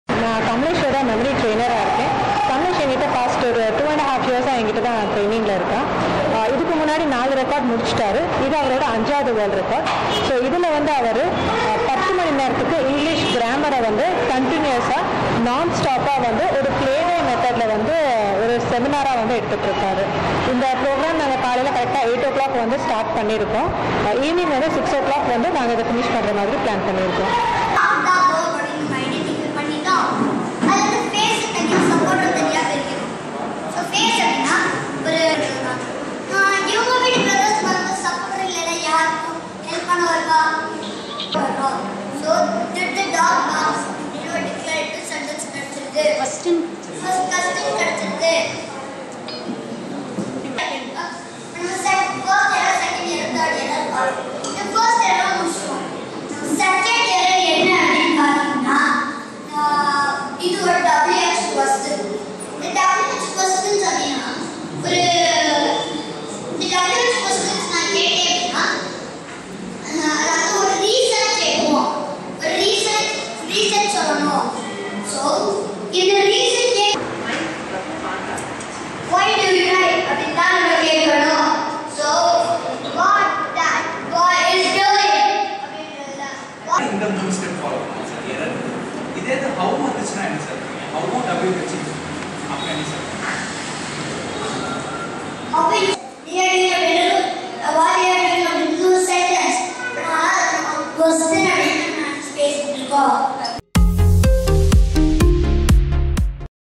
5 வது முறை உலக சாதனை பெறும் தேனியை சேர்ந்த 8 வயது சிறுவன்
5 வது முறை உலக சாதனை பெறும் தேனியை சேர்ந்த 8 வயது சிறுவன்